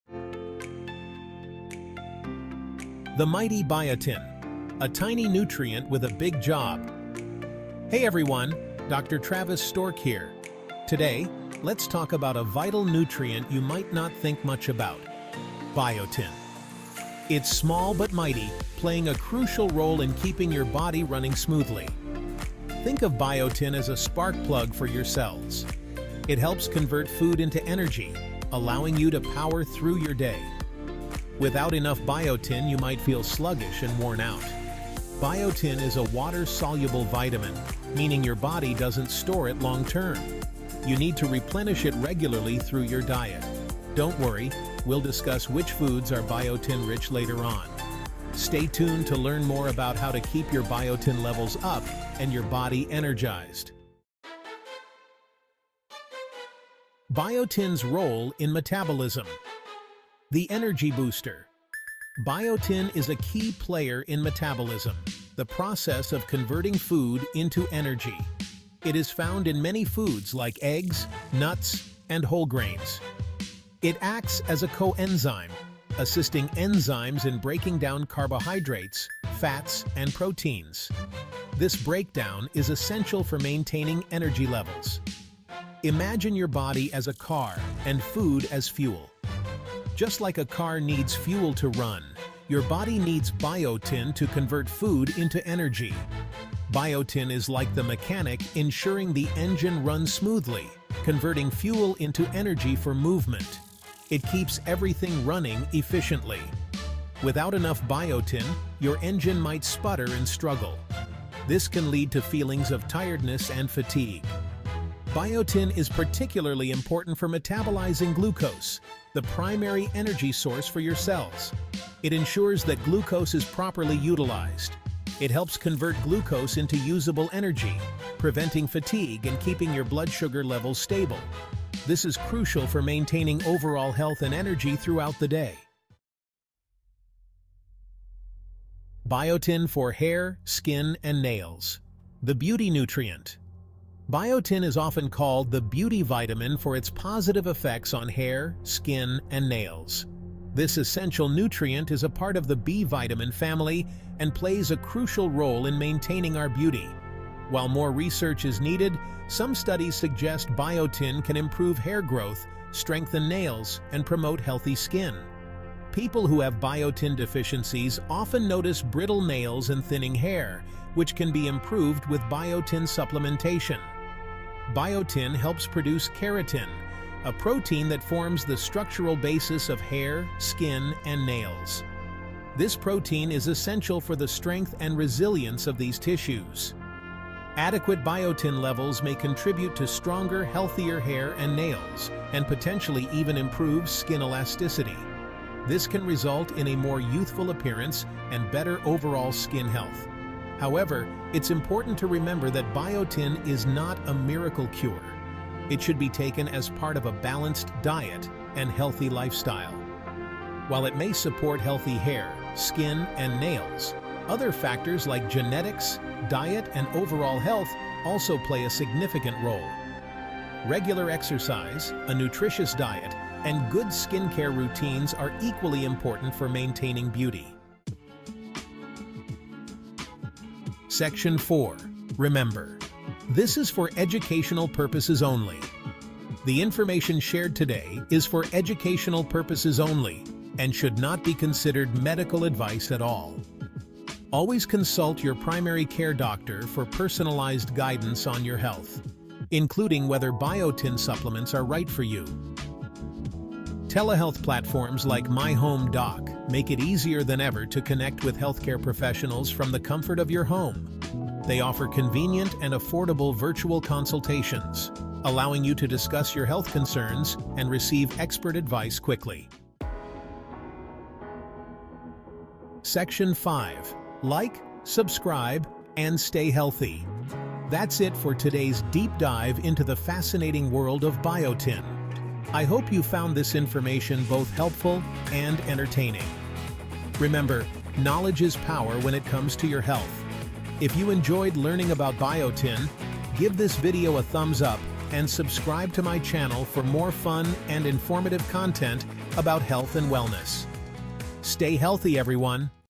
How Biotin works in your body and the benefits of biotin | MyHomeDoc Telehealth
How Biotin works in your body and the benefits of biotin | MyHomeDoc Telehealth                                                                                                                                                                           Welcome to MyHomeDoc Telehealth | Virtual Primary Care! In today’s video, we’re diving into the fascinating world of biotin, often known as Vitamin B7, and exploring its essential role in keeping your body healthy. This video will give you a complete overview of how biotin functions in your body and the amazing health benefits it offers.
In this video, you’ll learn:
How biotin supports energy production and contributes to various metabolic processes.
The role of biotin in promoting healthy skin, hair, and nails.
Biotin's impact on brain function and why it’s crucial for cognitive health.
Common signs of biotin deficiency and how to recognize them.
Whether you're curious about natural ways to support hair growth or looking for supplements that promote overall wellness, this video is perfect for anyone interested in maximizing their health through nutrition.